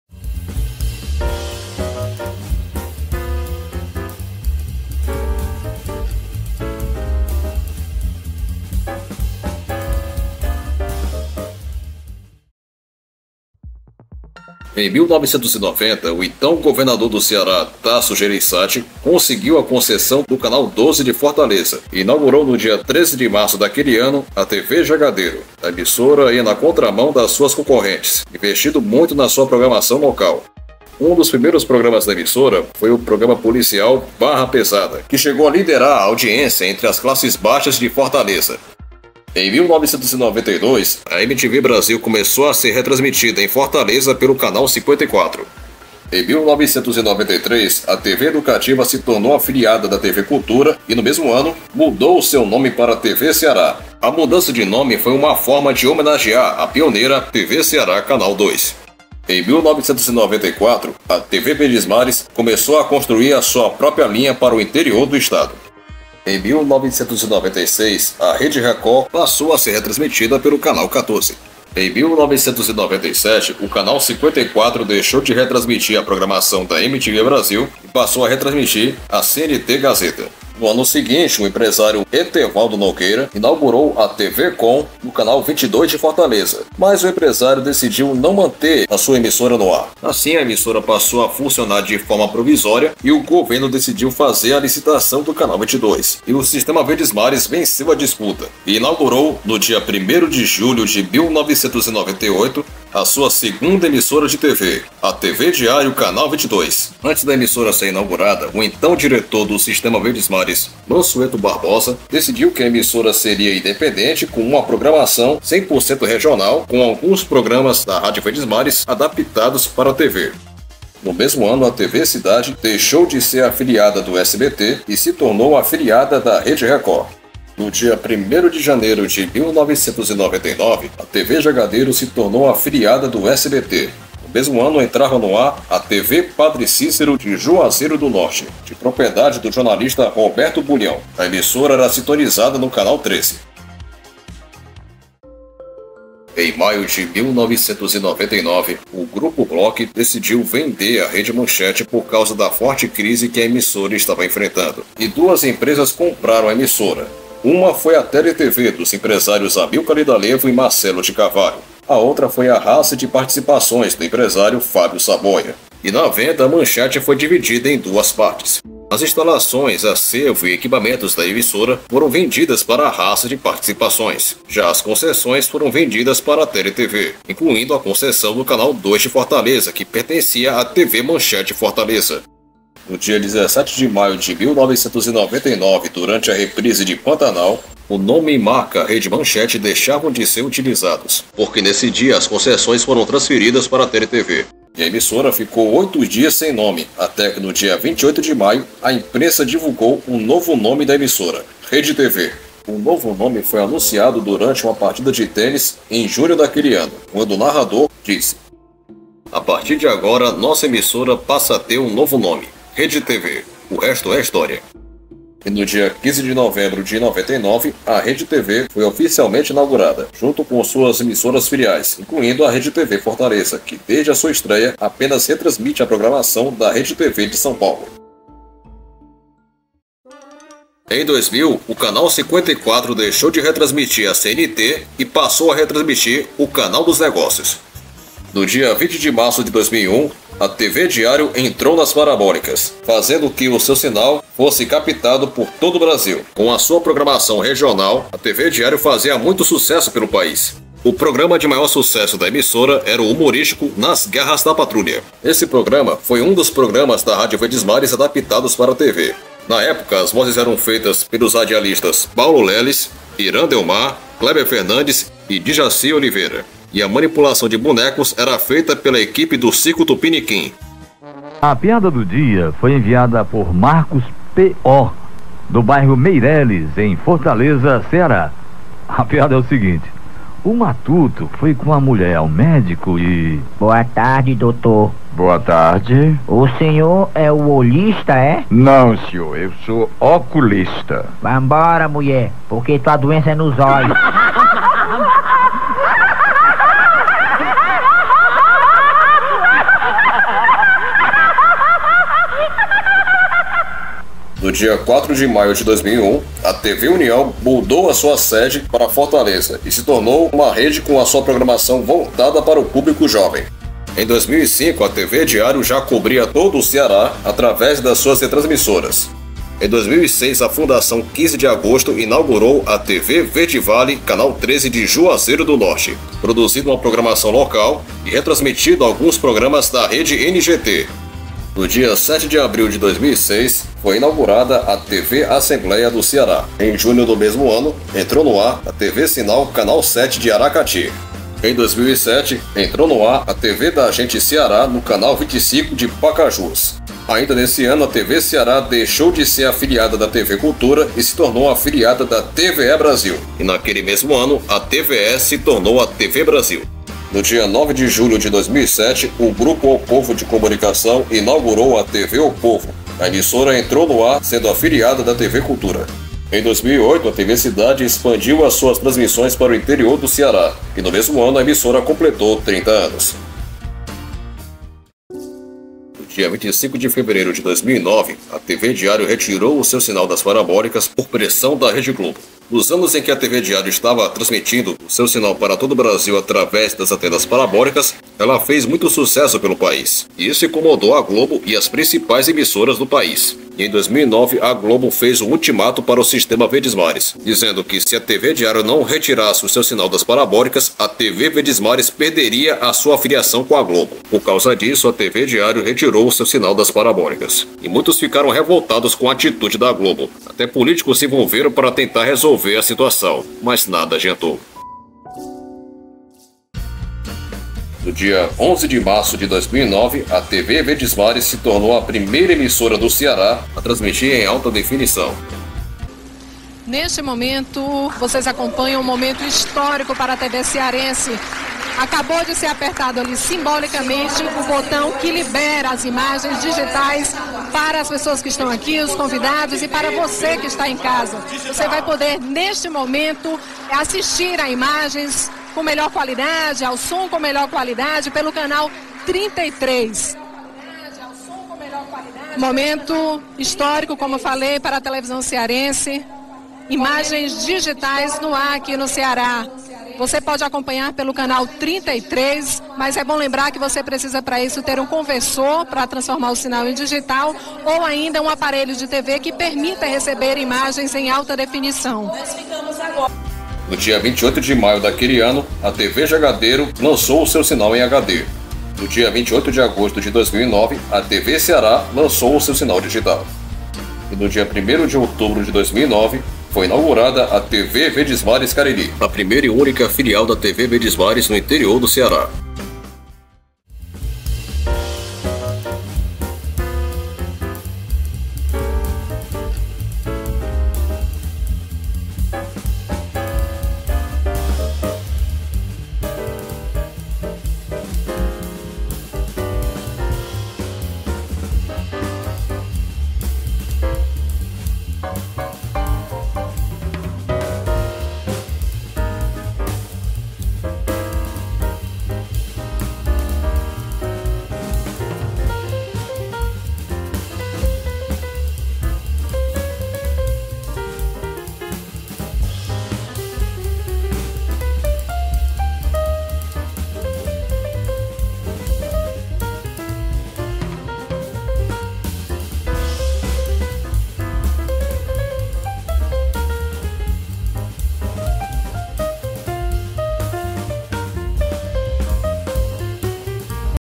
A Televisão no Ceará Parte 2: Decadas de 90 e 2000
DETALHES:
- Em 1999 a TV Ceará passou a retransmitir alguns programas da TV Senac.

- Em 2001 o canal 54 retransmitiu a TV Canção Nova ate 2003 quando passou a retransmitir a RIT.

- Em 2005 a Rede Gênesis entra no ar pelo canal 38 UHF.

-Em 2009 o canal 54 deixou de retransmitir a RIT e passou a retransmitir a Rede Mundial atravez da Rede 21.

- Em 27 de setembro de 2007, entrava no ar a Record News, que substituía a Rede Mulher tanto em Fortaleza quanto em outras praças que era retransmitida.

- O radialista Paulo Lélis morreu aos 56 anos na madrugada de 23 de janeiro de 2008, vítima de um infarto do miocárdio, enquanto dormia em sua casa e foi substituido pelo radialista Will Ferrari (que permaneceu no programa Nas Garras da Patrulha até 2018)

- O humorista Hiran Delmar saiu das Garras em 2009 e foi substituido por Ery Soares.

- O radialista e humorista Djacir Oliveira morreu no dia 8 de setembro de 2021 aos 81 anos por insuficiência respiratória aguda em decorrência da Covid-19.